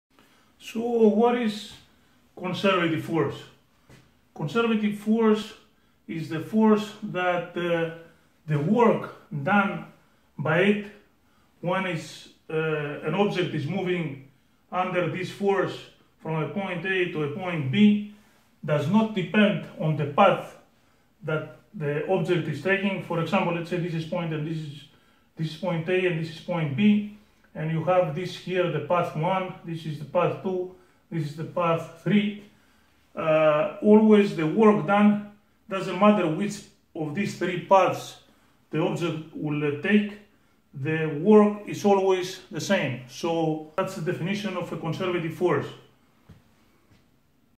Conservative force definition
George Lerakis gives the definition of conservative force. He says that conservative force is the force that the work done by it doesn't depend on the path taken by the object but only by it's initial and final position.